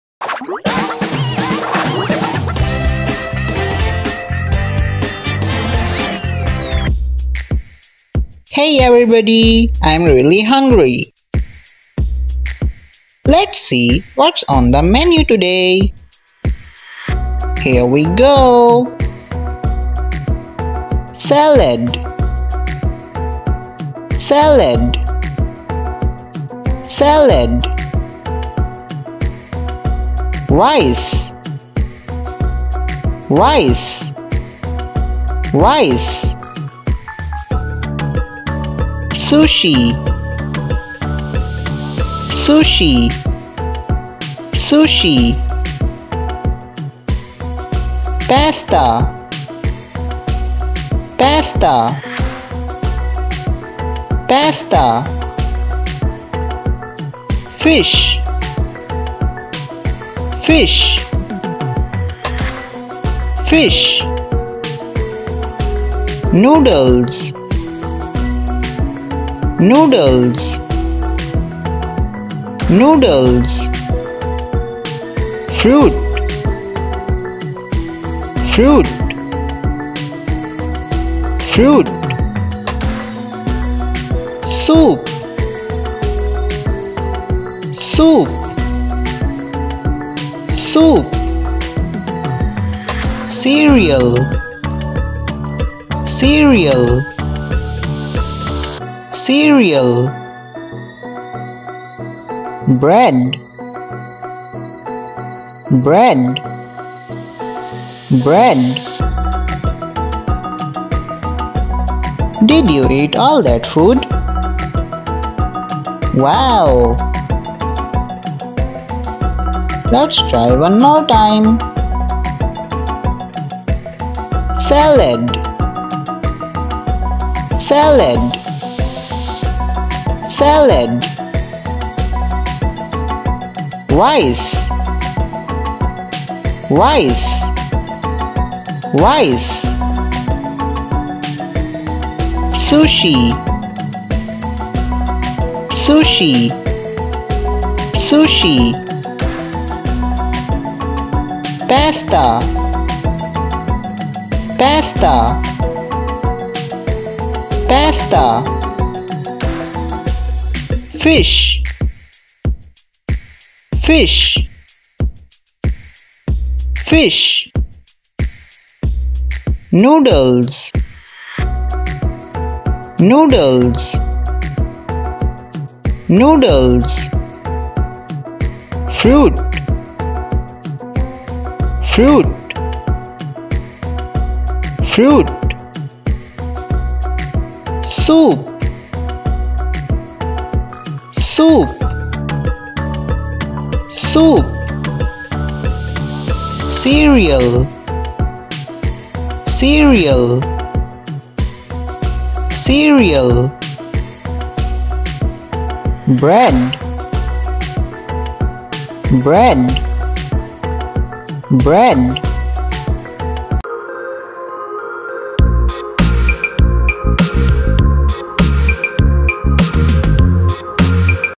Food Names in English | Kids Learning Food Vocabulary | ABC Food Names
Food Vocabulary in English

This video is parts to kids learning videos which includes learn A to Z food name in English, learn food vocabulary and food vocabulary words. And it is easy way to educate kindergarten to learn A to Z with food name for kids with fun and ease.